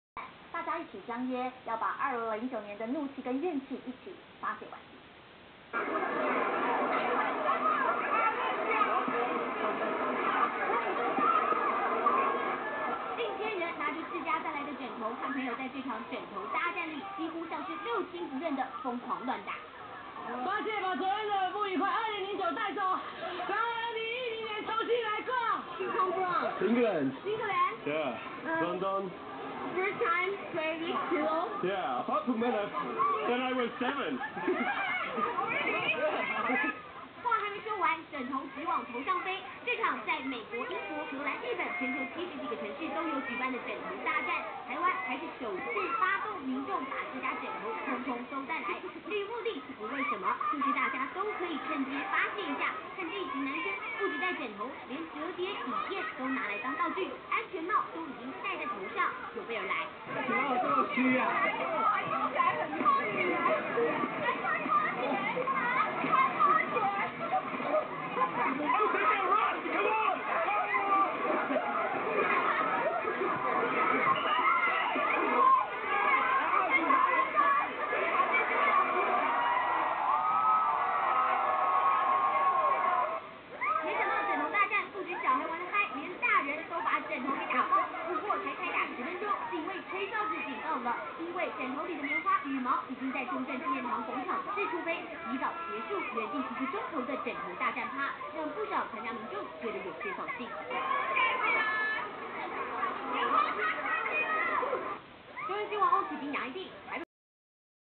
Taipei Pillow Fight news from Zhong Tien Xin Wen | 街頭枕頭大戰中天新聞報導
Jan, 02, 09, pillow fight :)